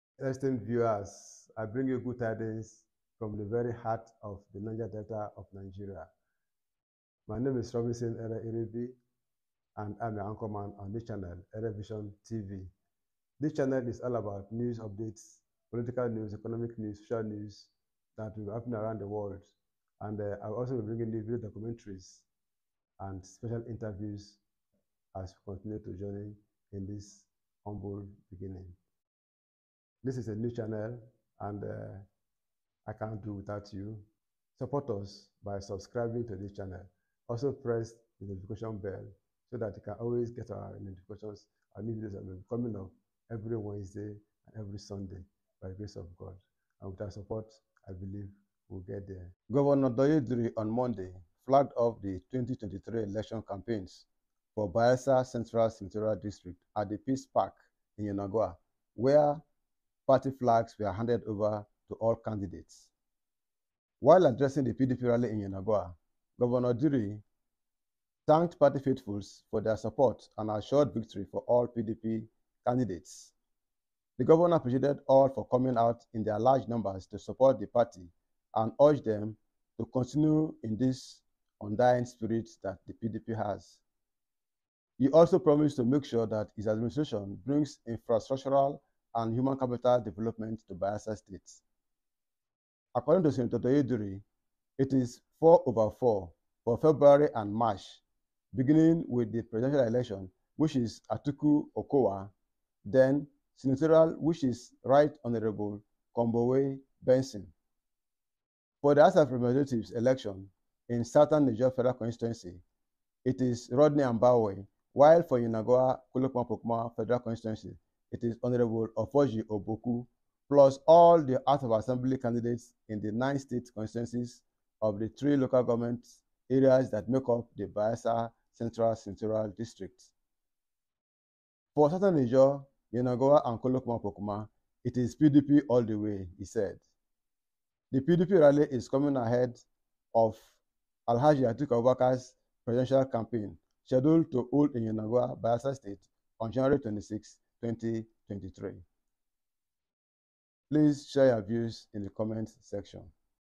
Sen. Douye Diri Flags Off Campaigns For 2023 Elections
PDP 2023 Elections campaign rally in Yenagoa on January 16, 2023